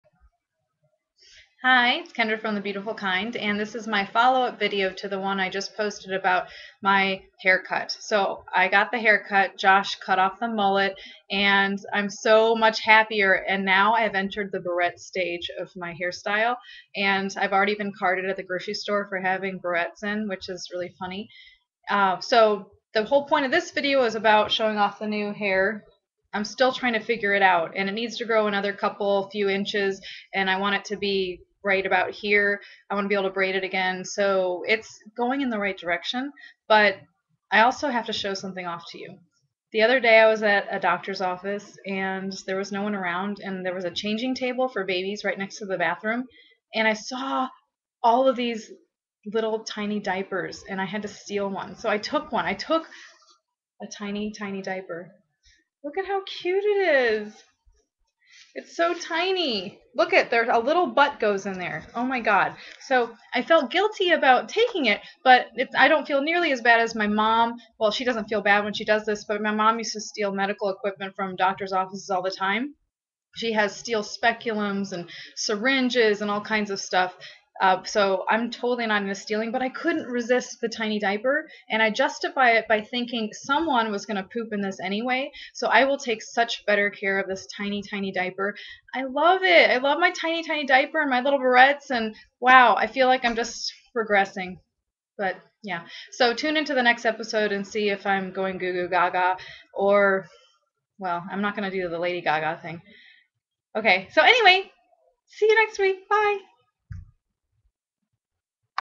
I Stole a Tiny Tiny Diaper
Kendra Holliday shows off her new haircut and confesses about stealing a tiny tiny diaper